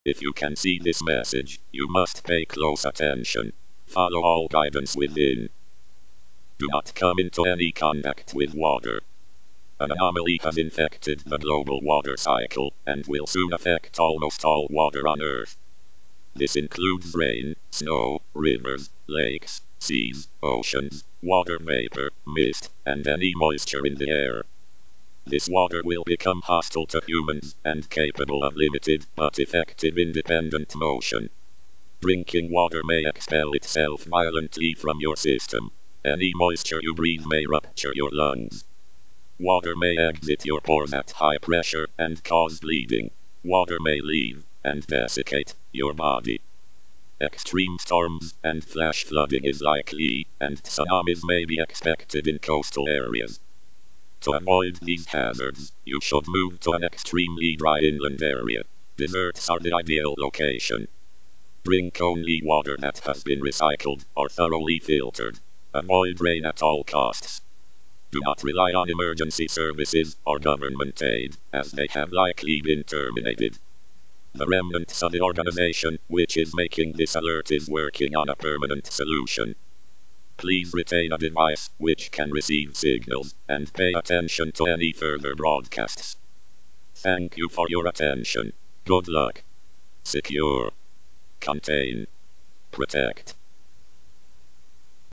AVOID ALL WATER! - SCP XK-Class Scenario Alert
Based on: "SCP-3280" by S D Locke, from the SCP Wiki.